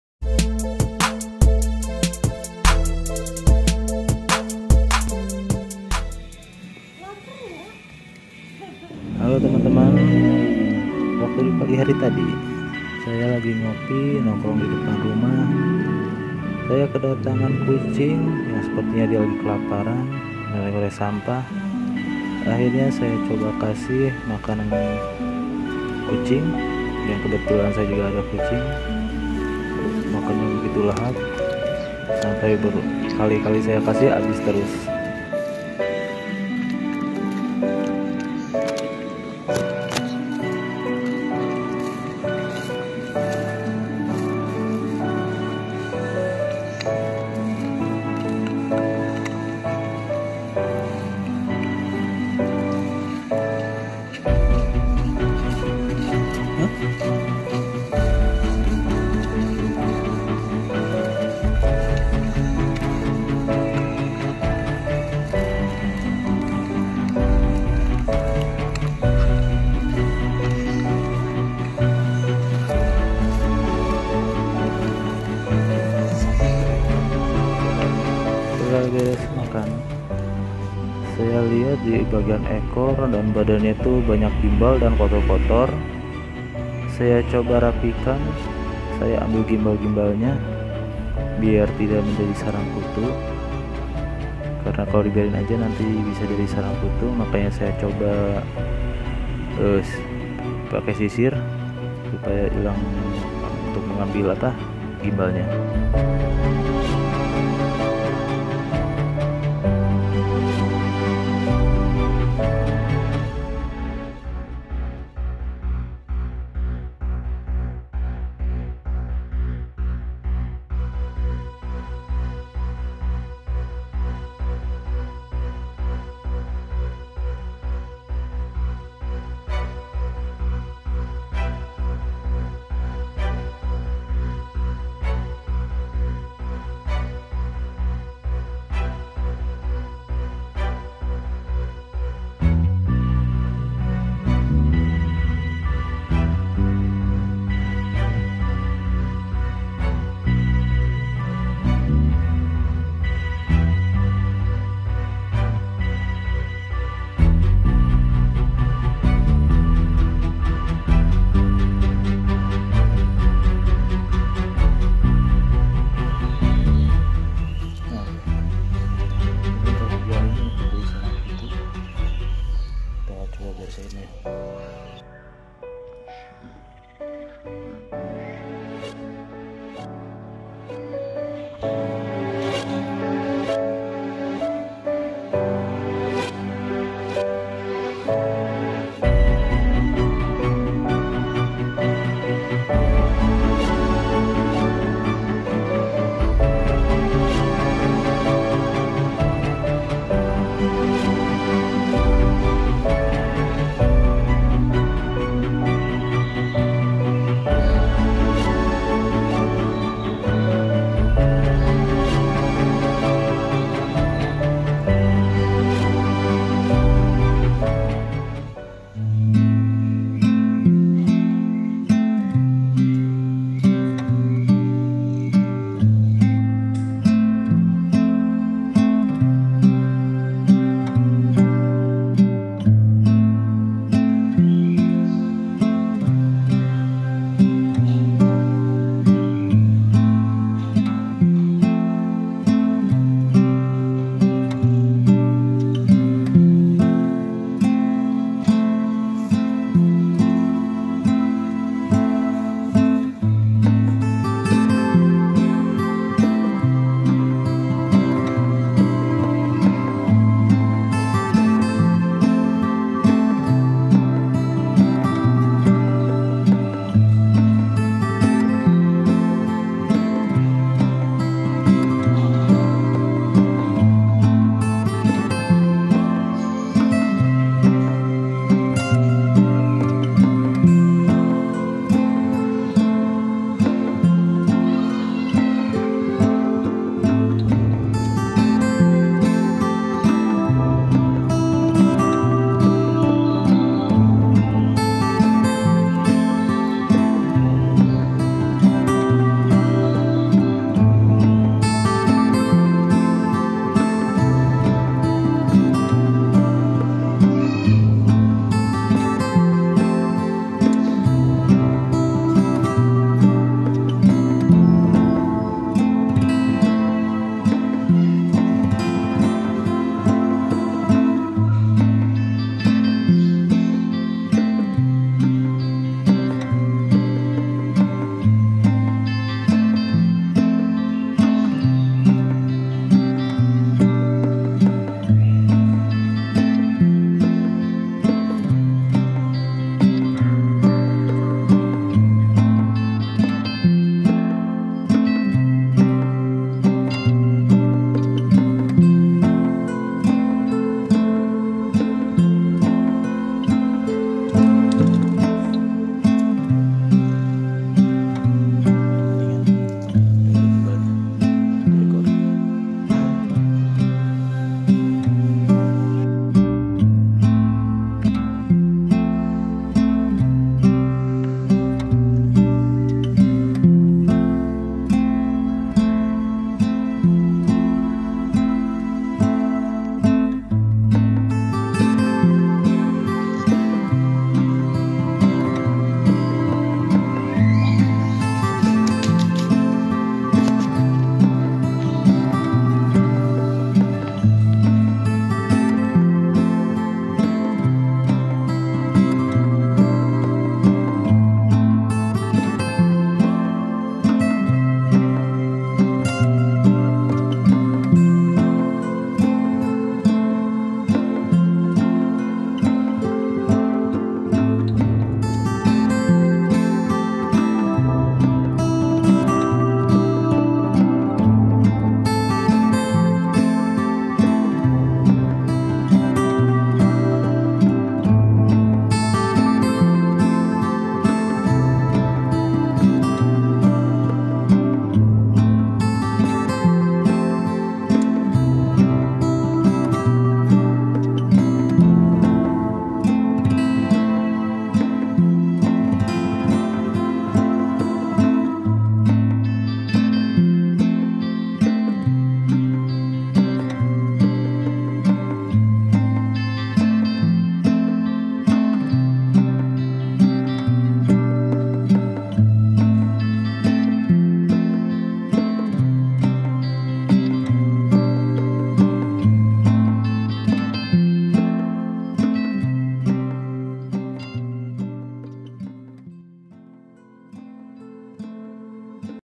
KEDATANGAN TAMU TAK DI UNDANG ⁉️⁉️⁉️
sayang sekali kucing sebagus ini terlantar sampai kelaparan.

andaikan di rawat dan dipelihara dengan baik cakep banget dah ini kucing.
dan ternyata kucing ini punya tetangga saya,ntah sudah gk sanggup merawat atau ntah gimana,bisa kaya gini kucingnya.